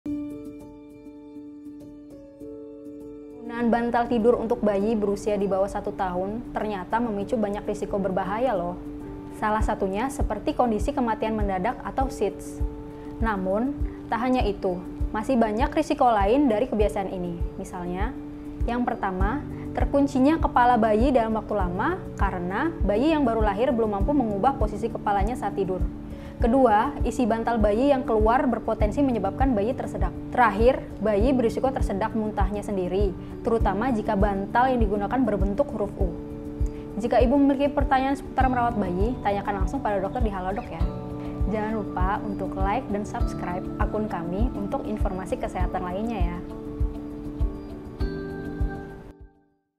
Risiko Penggunaan Bantal pada Bayi di Bawah 1 Tahun
Ternyata menggunakan bantal untuk bayi yang masih berusia di bawah satu tahun memicu banyak risiko yang terbilang cukup berbahaya. Bayi baru lahir tidak membutuhkan bantal sebagai alas kepala untuk tidur. Karena penggunaan bantal sangat berisiko menutup area mulut dan hidung bayi ketika tidur sehingga ia mengalami kesulitan bernapas.